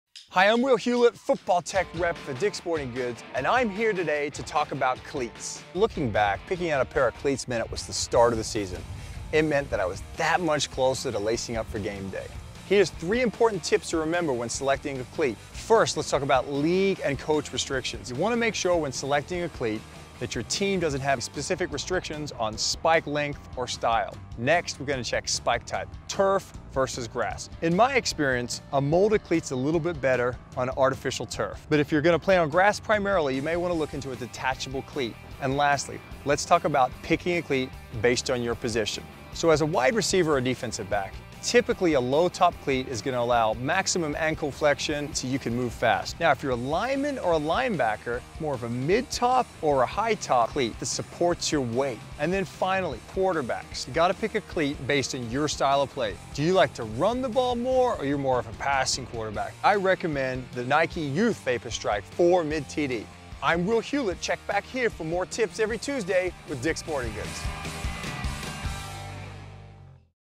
Football Tech Rep: Choosing The Right Cleats
Football expert and coach Will Hewlett breaks down the various types of cleats to help you make the right decision for footwear on the field. A number of factors could impact your choice based on league restrictions, playing surface, and position.